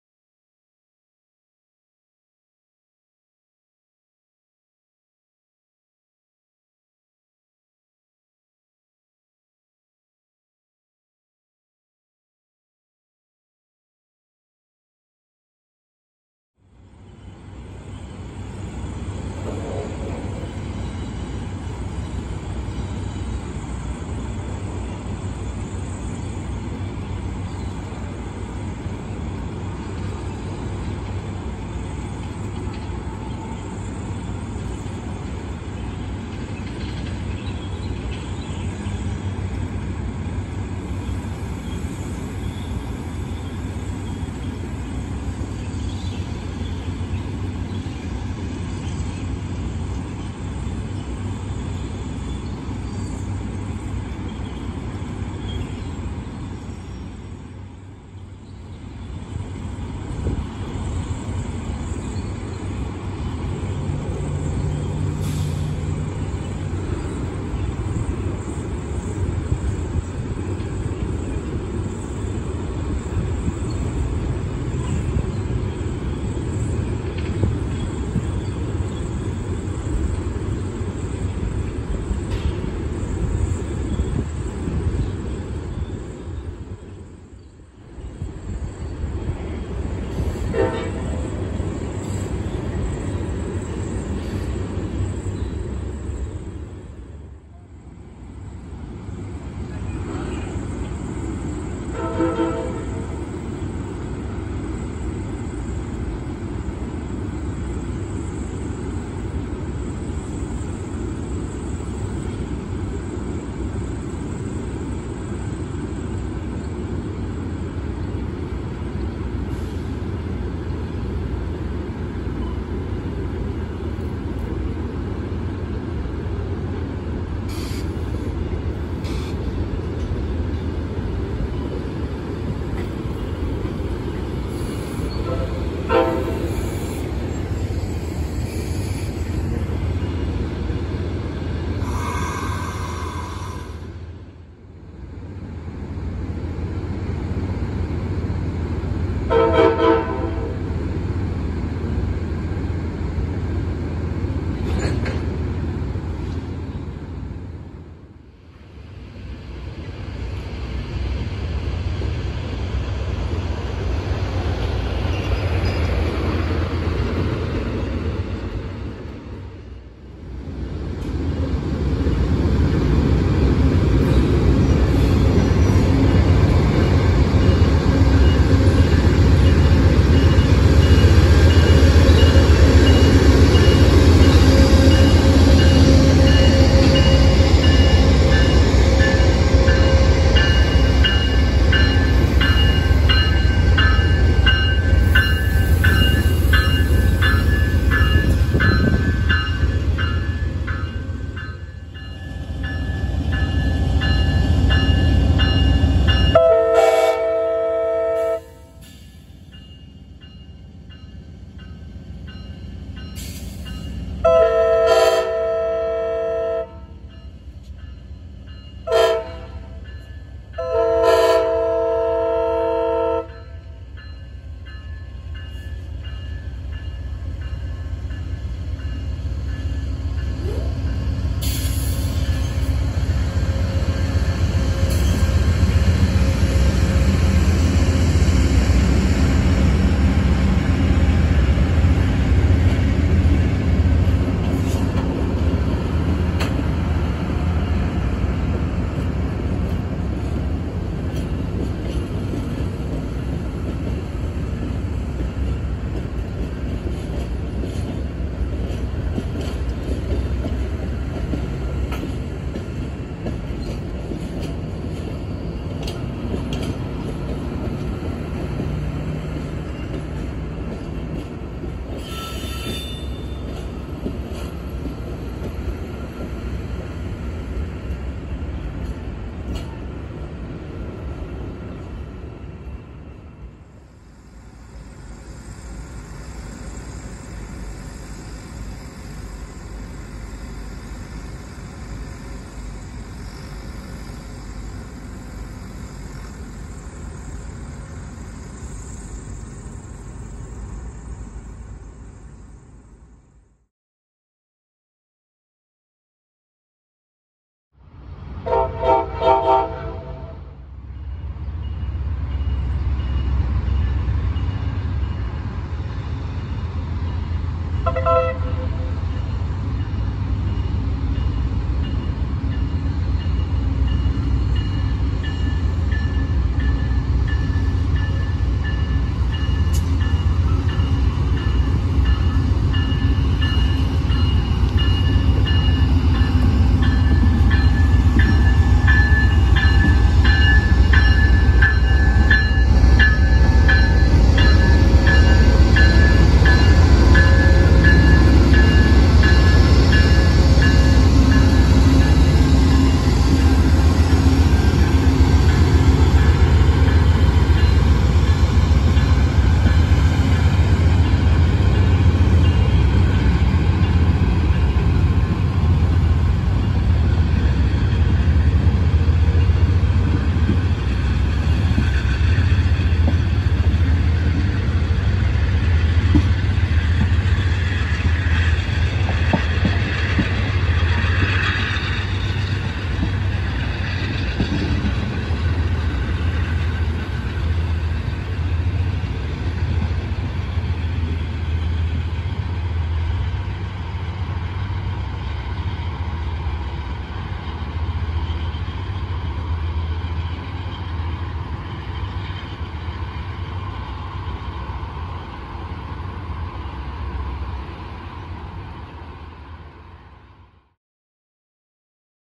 Amtrak Texas Eagle with Phase VII Leader at Fort Worth, TX (June 13, 2024)
We are out along E. First Street near downtown and the Purina Mill. Here we see the Texas Eagle as it turns around on the wye. Due to weather related events that affected the Mineola Sub between Dallas and Longview, Fort Worth was the final destination for this train. For locomotive power, P42DC #138 (with Phase VII paint) was on the point, followed by #68 which was trailing behind. After a 30 minute stop at Fort Worth Central Station, we would catch the Eagle as it headed back to San Antonio. Note the horn acknowledgment and the engineer waving as the train cleared Tower 55.